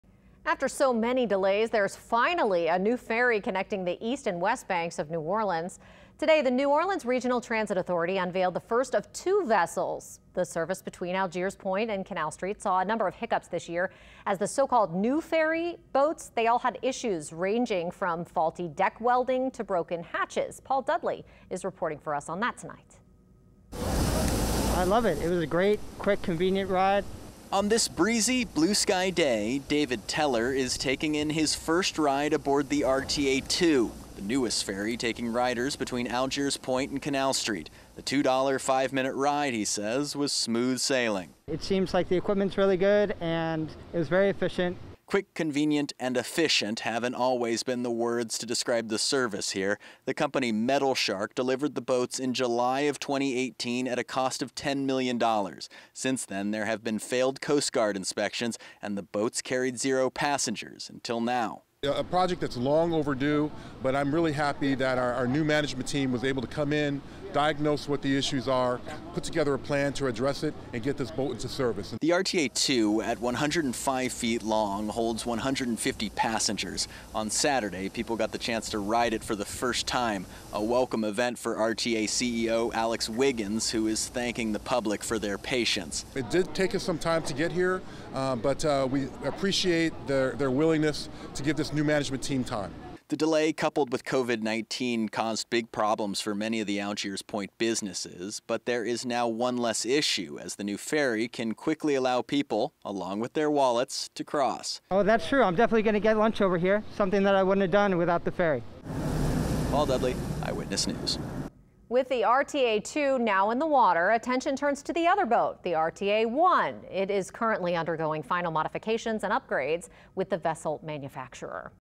More than 2 years after ferries delivered, New Orleanians ride pedestrian ferry in 'maiden voyage'
in July 2018, the first of two sleek new ferry boats arrived in New Orleans to much fanfare and hope. They were slated to replace the aging and unreliable boats carrying passengers across the river between Canal Street and Algiers Point.